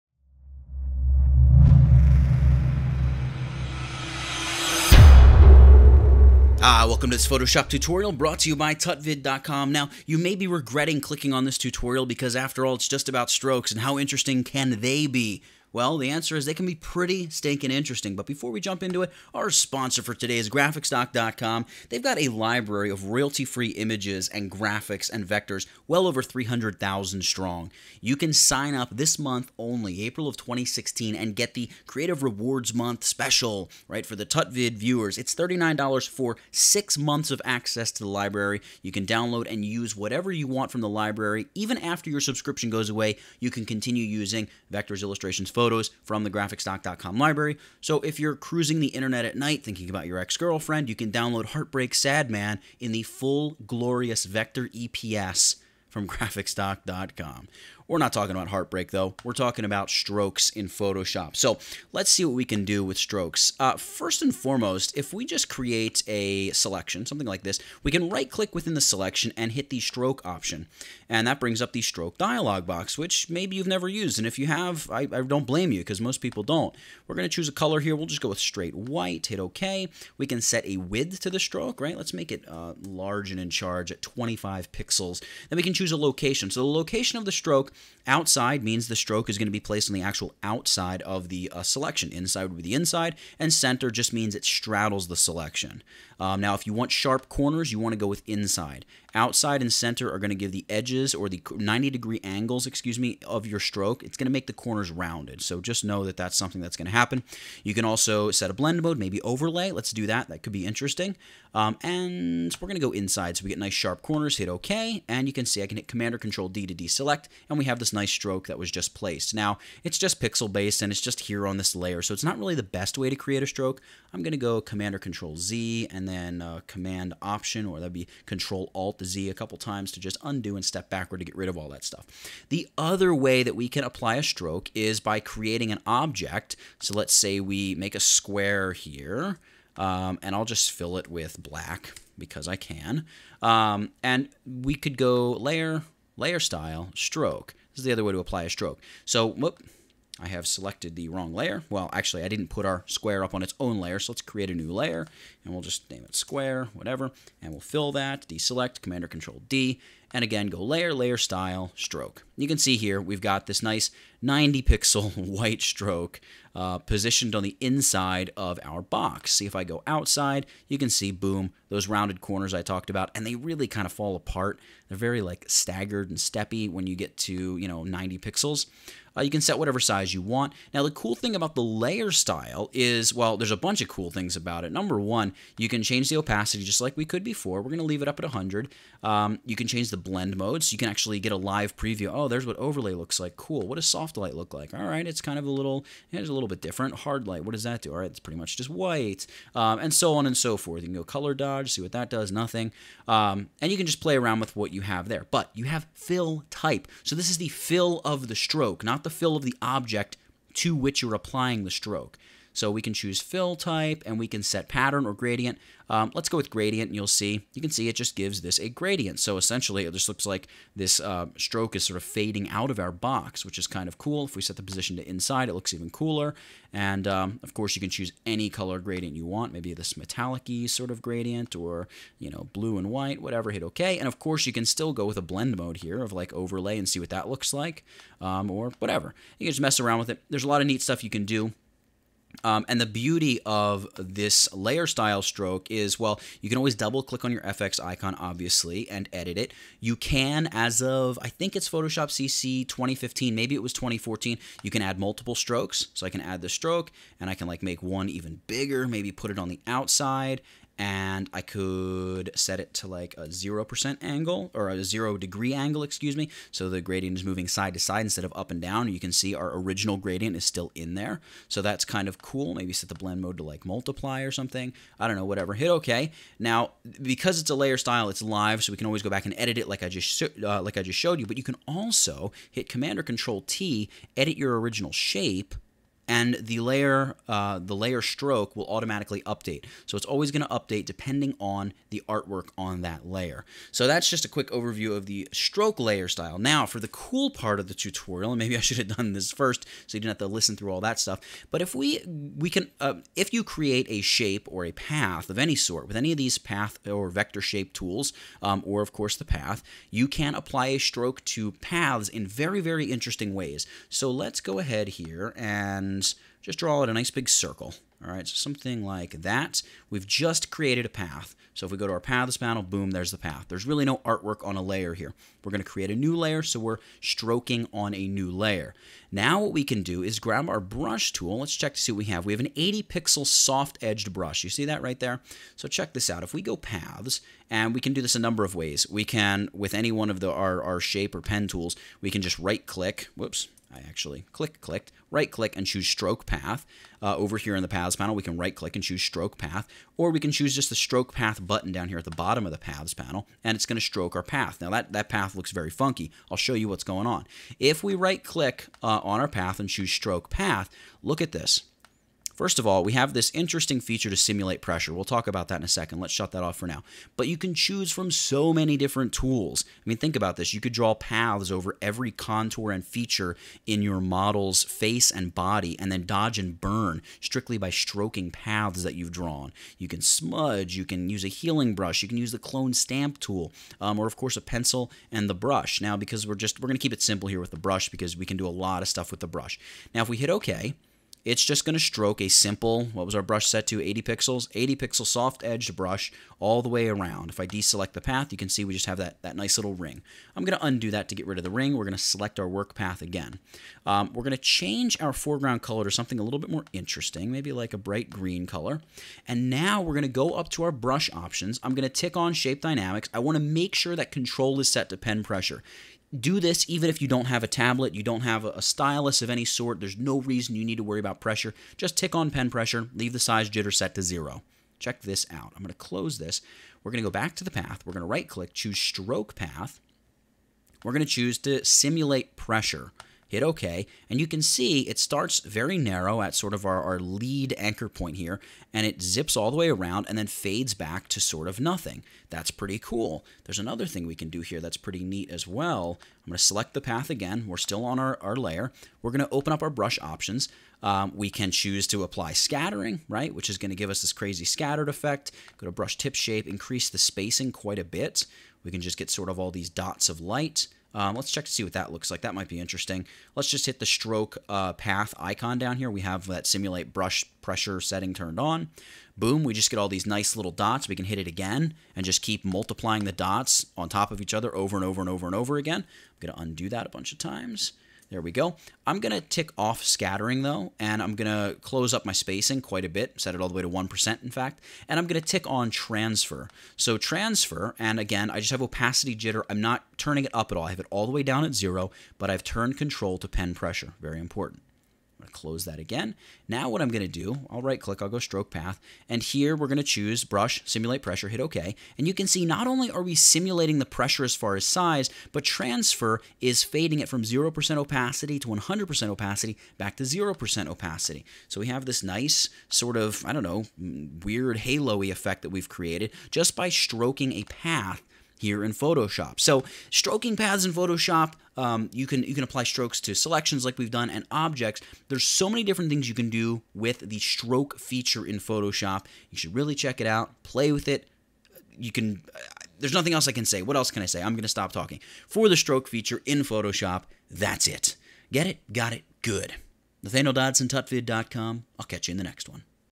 All About the STROKE Feature - Photoshop CC
Much like my tutorial on the Fill feature in Photoshop, the Stroke feature is also a tough one to get excited about, that is until you actually start playing with some of what you can do with strokes in Photoshop. Sure there is the Layer Style stuff (and I’ll cover that in this tutorial,) but when you start mixing brushes, paths, and brush options, you can get some really, really cool effects. This tutorial is an absolute must-watch for anyone who likes cool stuff and likes Photoshop.